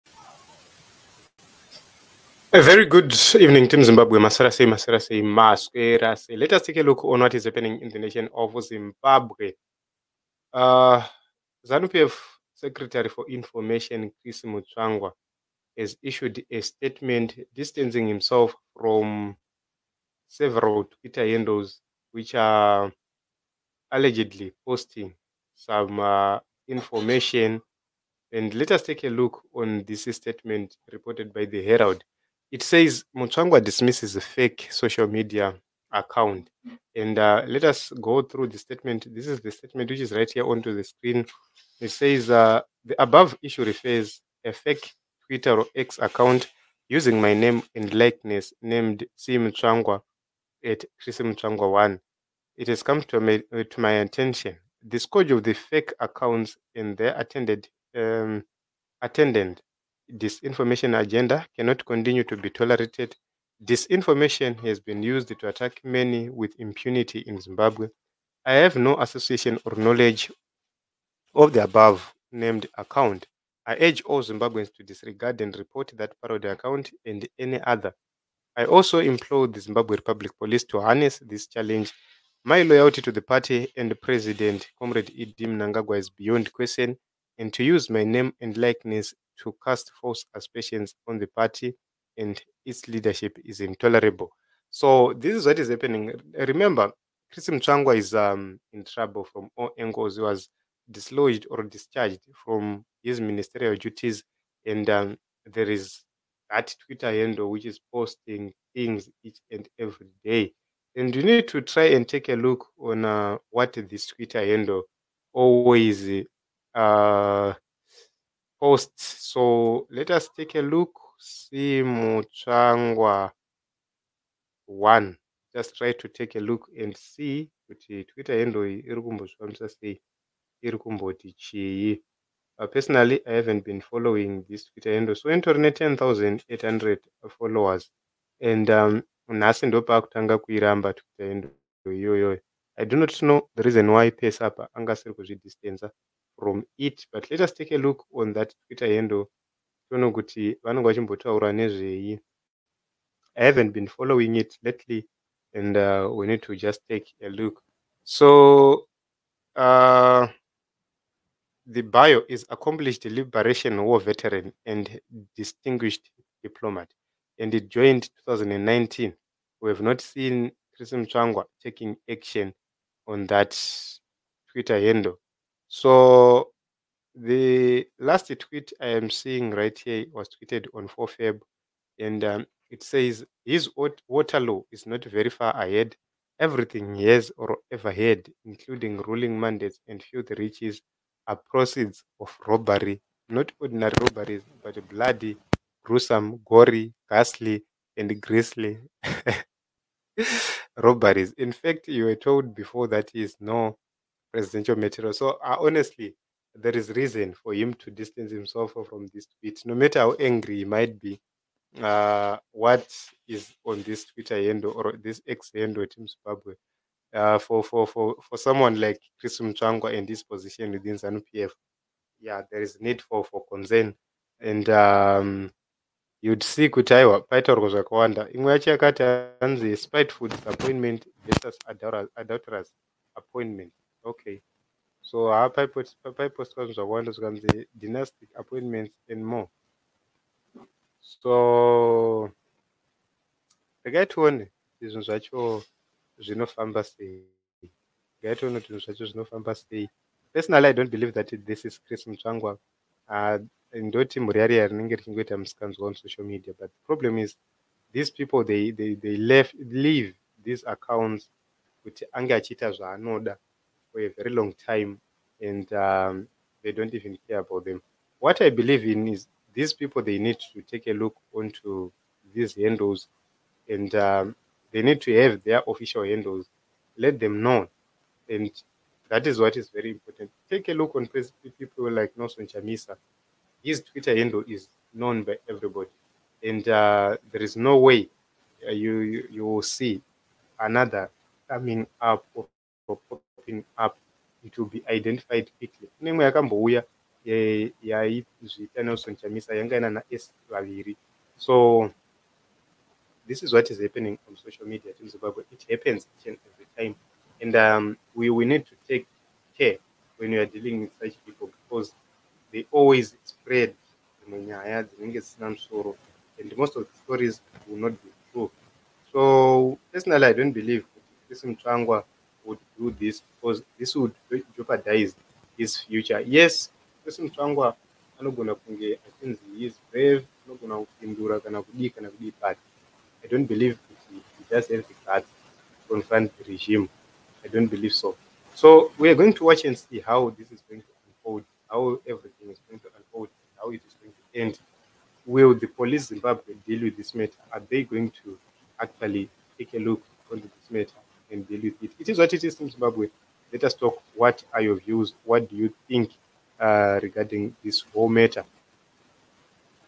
Troubled Mutsvangwa DISTANCES himself from X account blasting Mnangagwa
Mutsvangwa distances self from toxic X handle